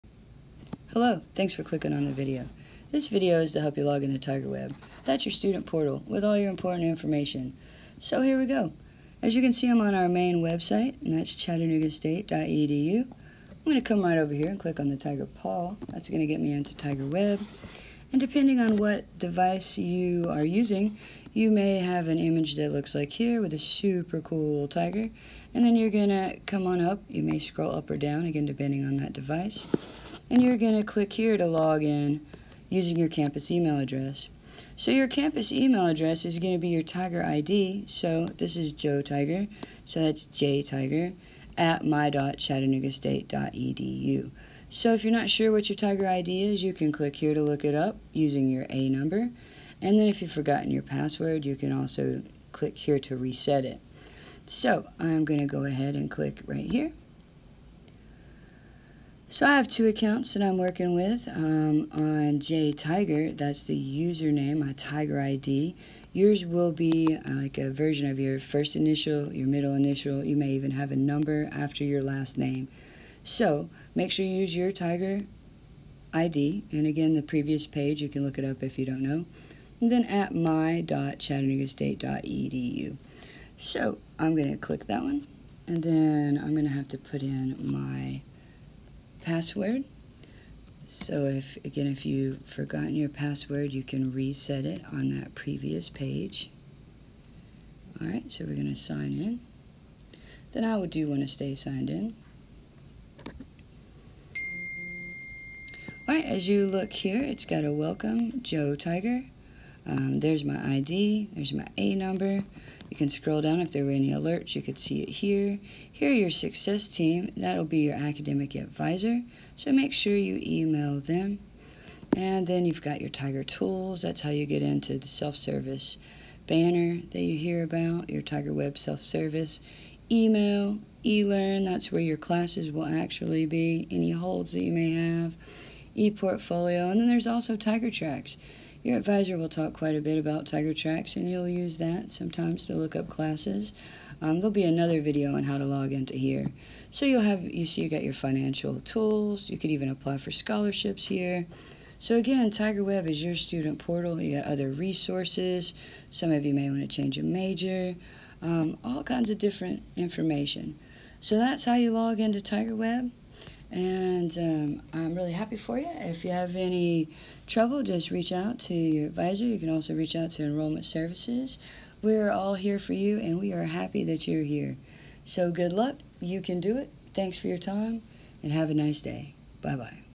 How to log in to TigerWeb at Chattanooga State
TigerWeb is Chattanooga State's student portal.
Log in to TigerWeb at tigerweb.chattanoogastate.edu

Apply to ChattState at chattanoogastate.edu/admissions